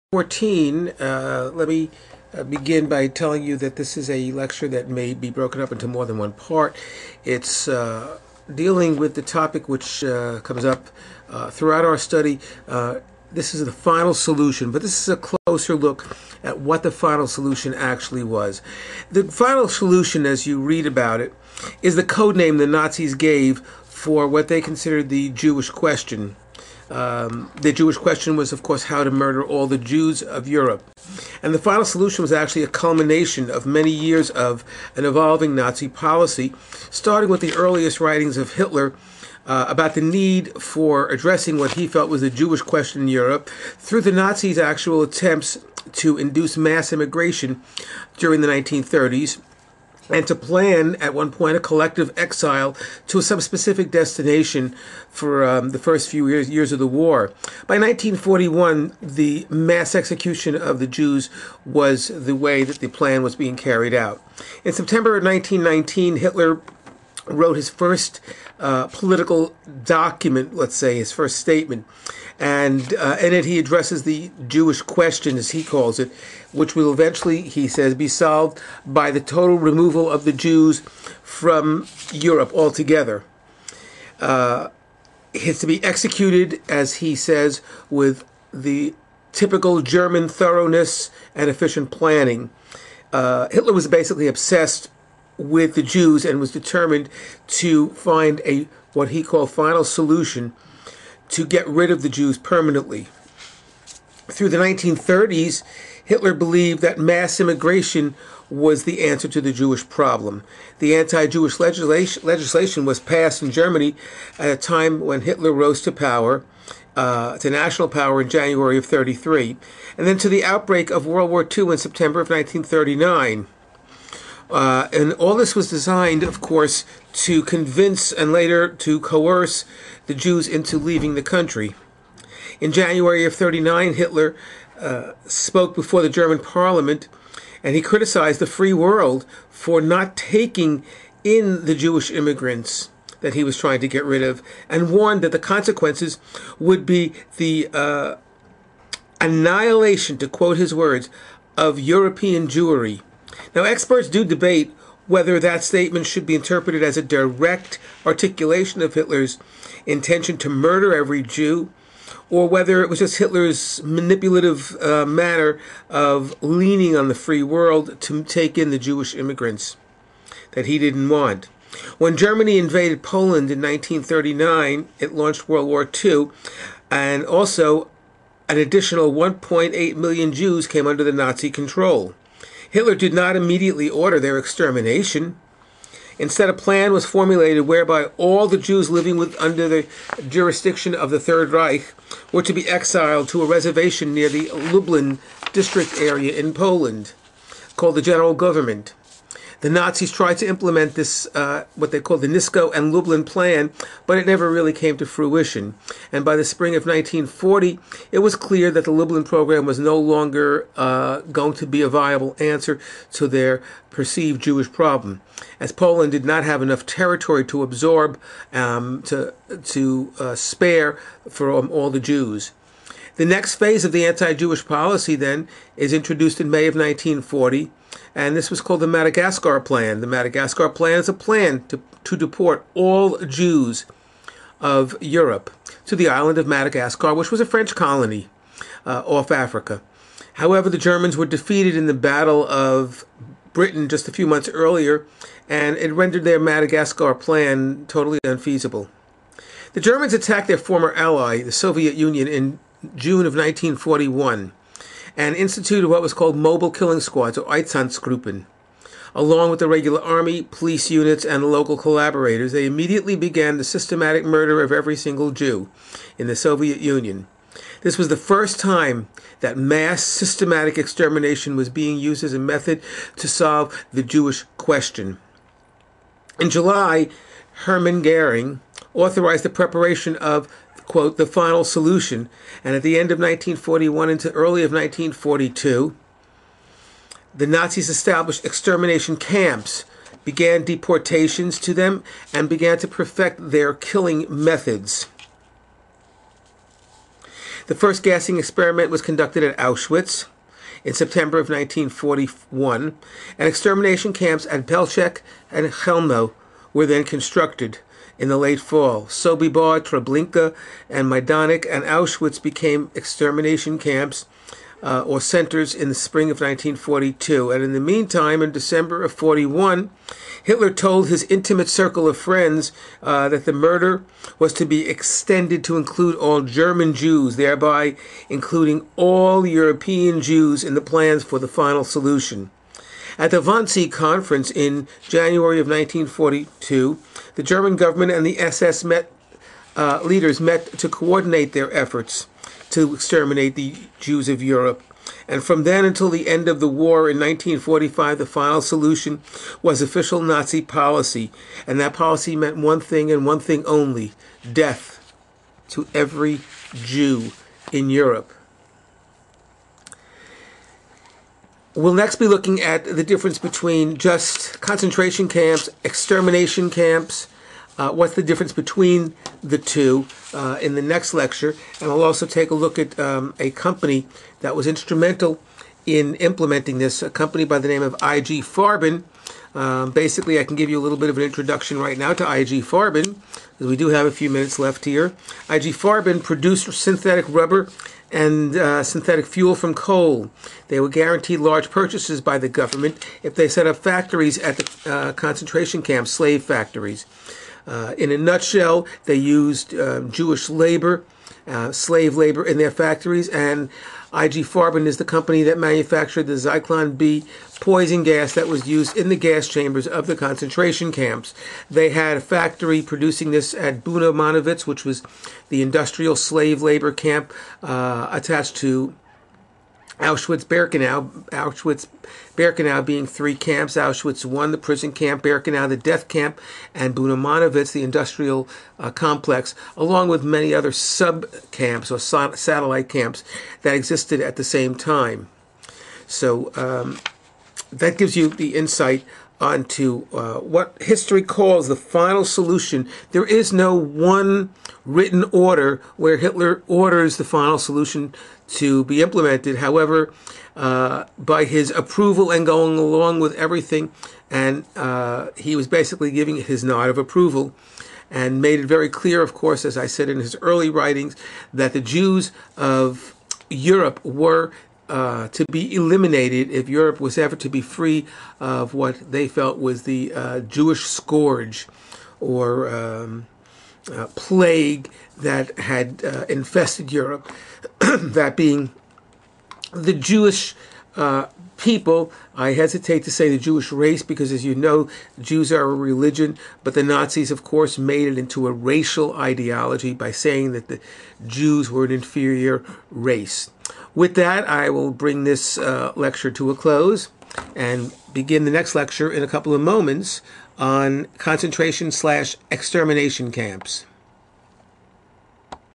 Lecture 14- The Final Solution
Overview of what the Nazis termed "THE FINAL SOLUTION" to the Jewish question.